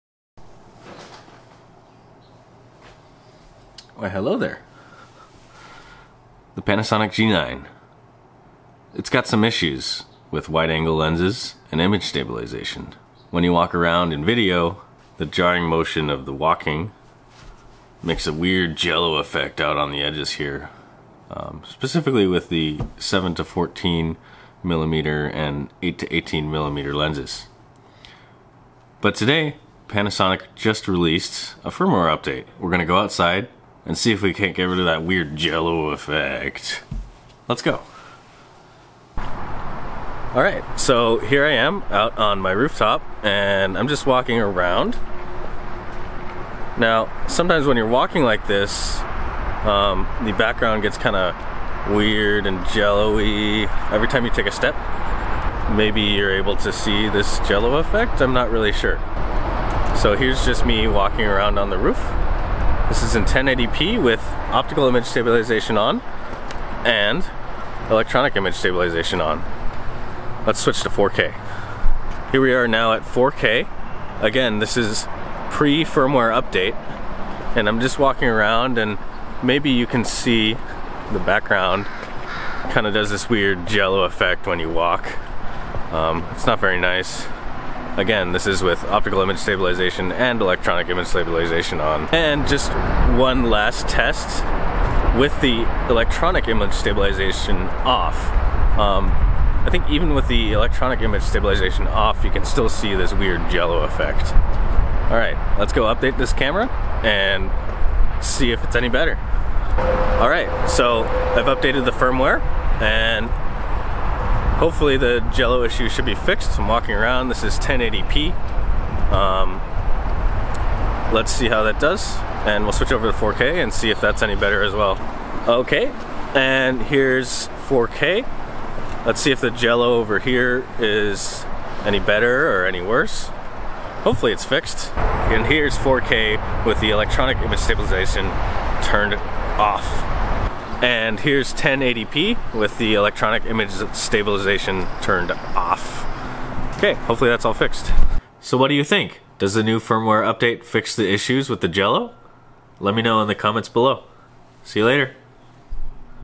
Panasonic G9: Stabilization while walking - Firmware Update
Panasonic DC-G9, May 30, 2018 Firmware version 1.1

"2. Improvement of Body I.S.(Image Stabilizer) performance

There were cases where peripheral distortion occurred in video recording while walking when used with Panasonic wide zoom lenses (H-F007014, H-E08018). This bug has been fixed."

Flor vlogging the 8-18mm gives a great wide field of view, but the background can get weird distortion from the IBIS. The jello effect is most noticeable with straight lines, watch the neighboring buildings to see the wobbling. With the new firmware and EIS turned on, I see significant improvement. What do you think?

Shot on:
Panasonic G9
Panasonic Leica 8-18mm
Pistol Grip / Tripod

Using the links above helps me out without costing you an extra penny. Thanks!